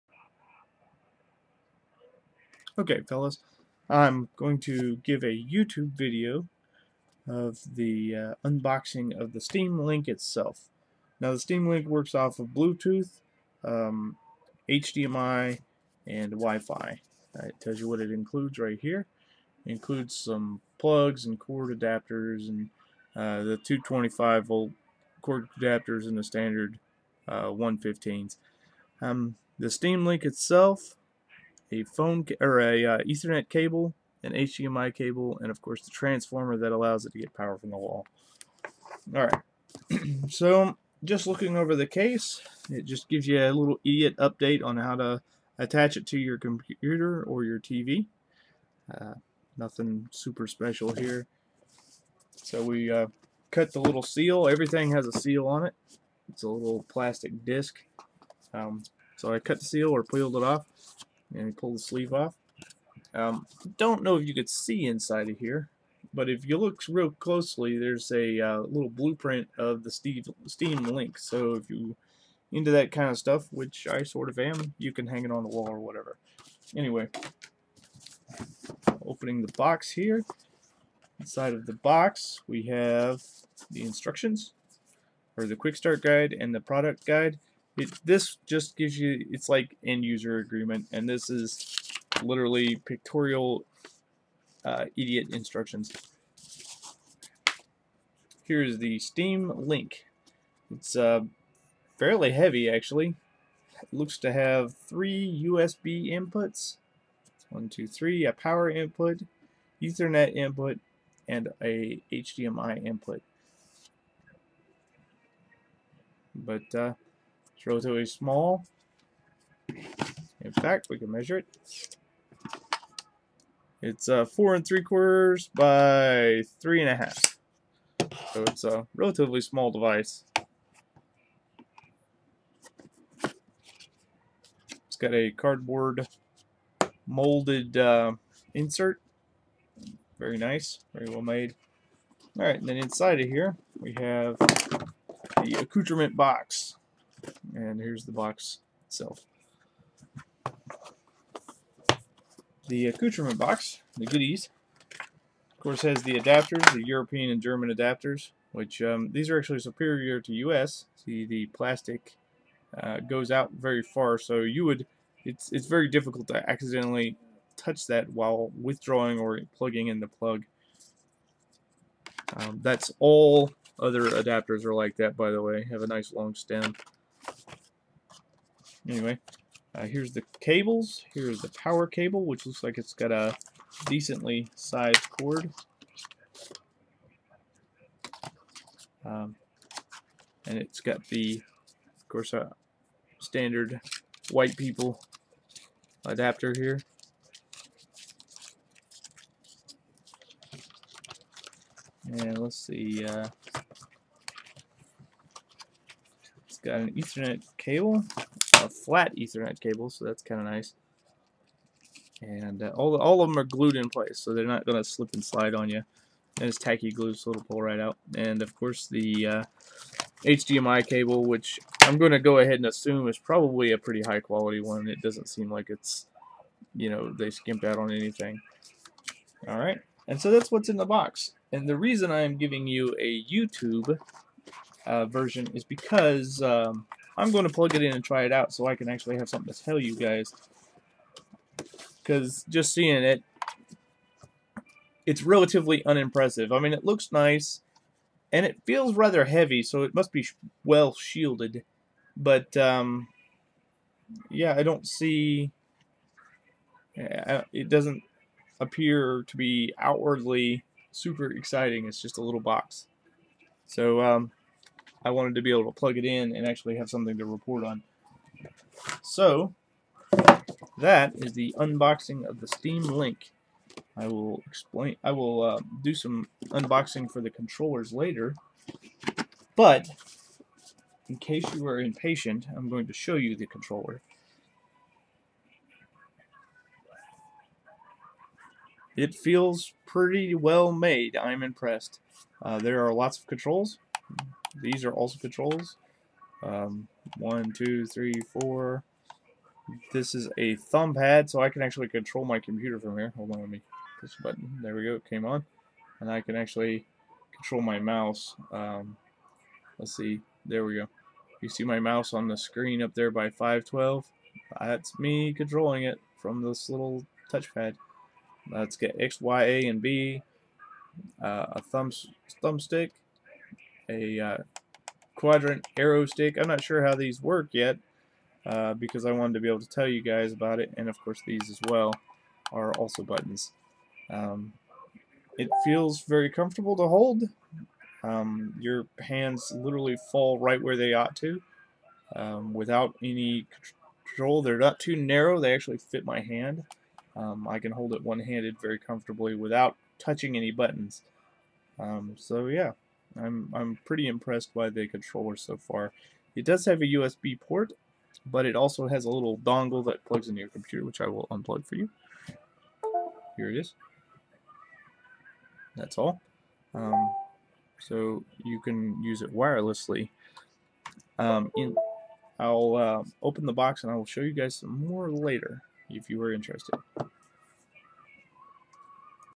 Steam Link unboxing
Sorry to unbox in a non-live session, but I wanted to plug it in and try it out so I could have something to actually report on.